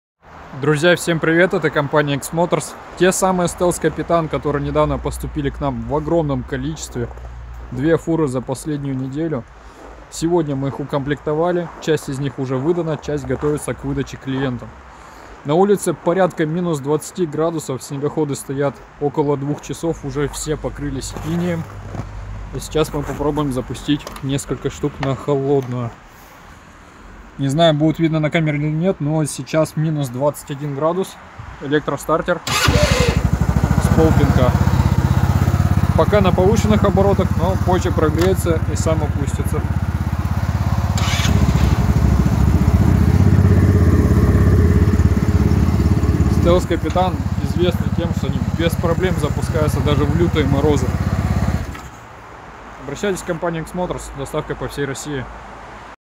ХОЛОДНЫЙ ЗАПУСК снегохода STELS Капитан 150/200
Поступление снегоходов СТЕЛС КАПИТАН 150 и 200 на короткой и длинной базе в X-MOTORS!
В наличии сток, доработка в 200 куб.см, XE (X-MOTORS EDITION) и многое другое доступно для установки в нашем Сервисном Центре.